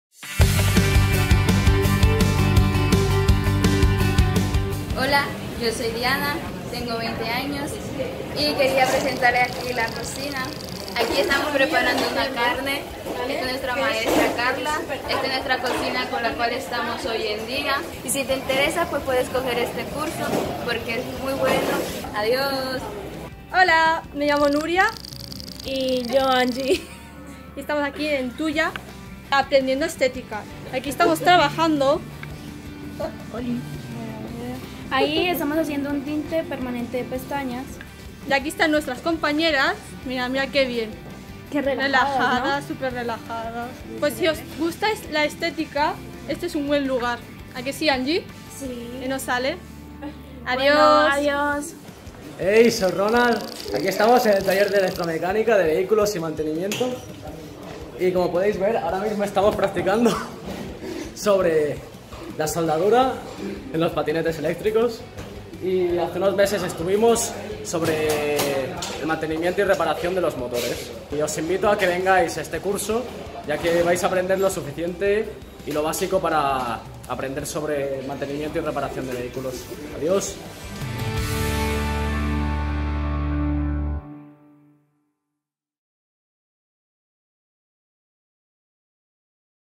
Forma't amb Barcelona Activa! - Formació per a joves
Si vols millorar el teu perfil professional, descobrir de què pots treballar, ampliar els teus coneixements, dominar les últimes tendències tecnològiques o aprendre una nova professió, vine a Barcelona Activa i t'ajudarem a marcar la diferència amb el currículum!